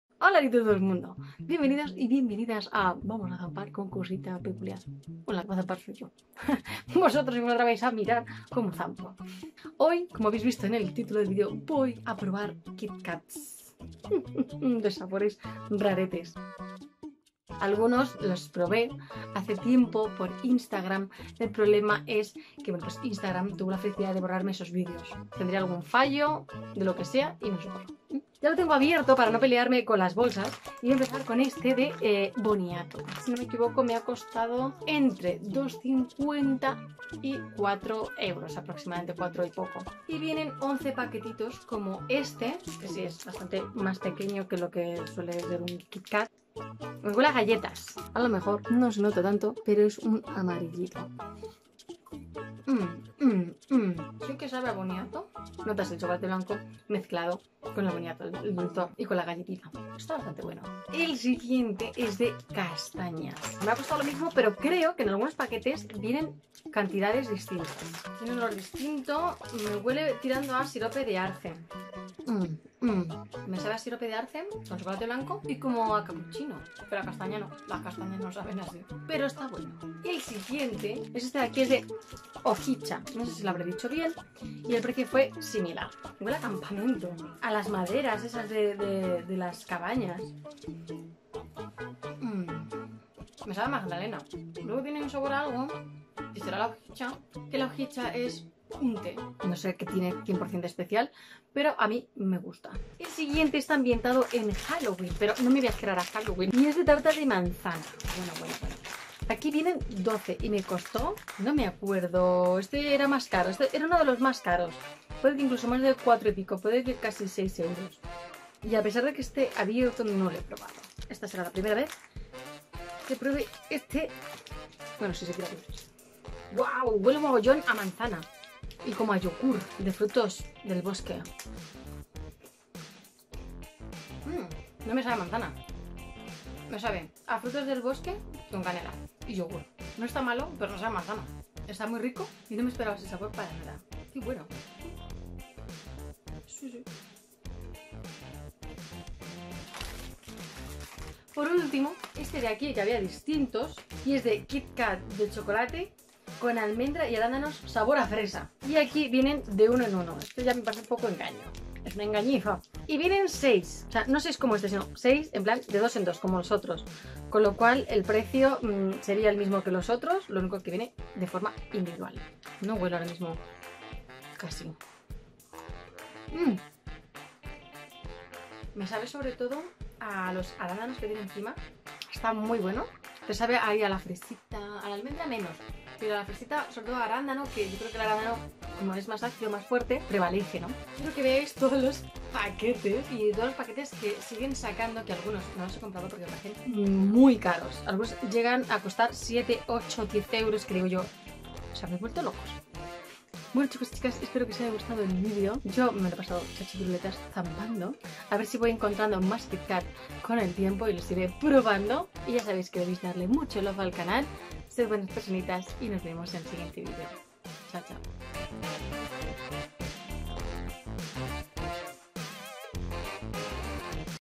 Probando KitKat 🍫JAPONESES RAROS🎌 ll Cosita Peculyar
¡Hola Personitas! Hoy voy a probar cinco de los famosos chocolates en barras de la marca Kitkat de Japón , he encontrado algunos sabores raros y exóticos ¿Serán tan curiosos estos snacks de Kit Kat? ¿Tienen buen sabor? ¿Es barato o caro? Echa un vistazo al vídeo y averígualo 😉 y si quieres que pruebe más chocolates de KitKat, deja tu love en comentarios y si llegamos a 100 Likes subo un segundo vídeo probando Kit-Kat raros. Compritas hechas en tiendas Asiáticas y por la web de Japonshop (no patrocinado).

(Todas las canciones son adquiridas en la biblioteca de música de Audio de Youtube sin Copyright, estas de aquí no requieren atribución, pero de igual forma dejo constancia).